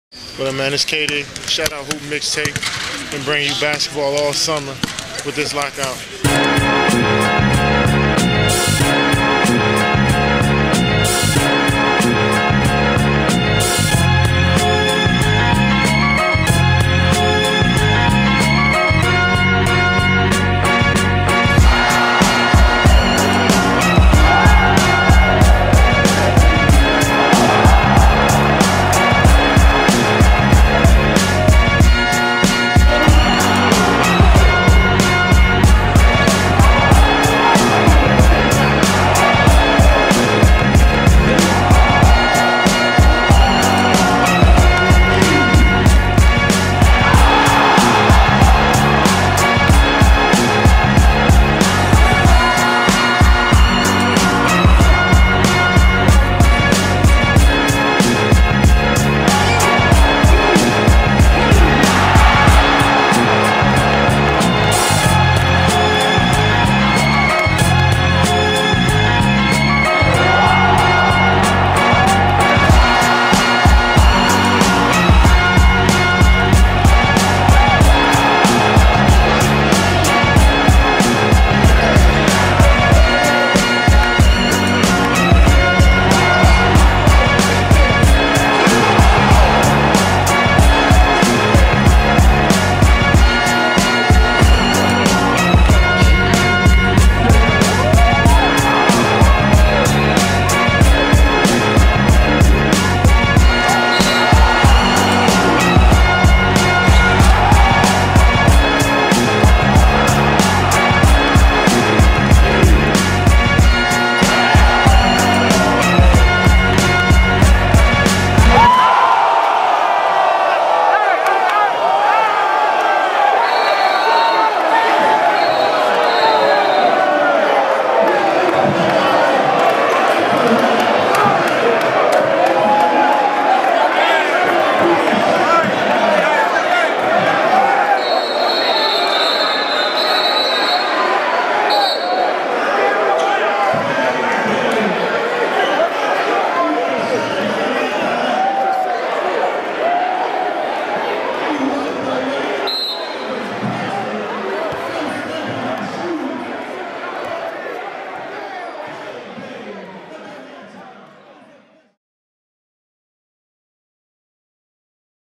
Kevin Durant OFFICIAL Lockout Hoopmixtape!!! The MVP Of The Lockout!!!
Here is Kevin Durant's Lockout Hoopmixtape. The Durantula traveled more in the lockout than in an NBA season, hitting up almost every major park, league and all star game there was. More of Kevin Durant coming soon on Hoopmixtape.com!